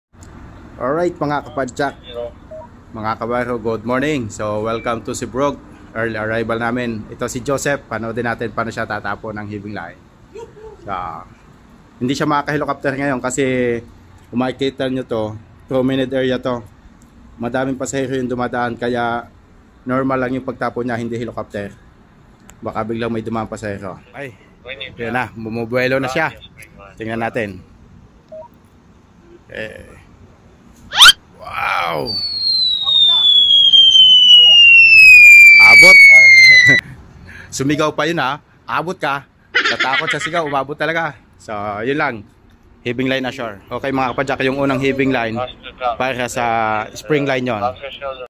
HOW TO THROW HEAVING LINE |CRUISE SHIP
Vlog title: HOW TO THROW HEAVING LINE | CRUISE SHIP
Alright mga Kapadyak.. This is the basic way to throw heaving line, because of the limited Space they cannot throw Helecopter Style.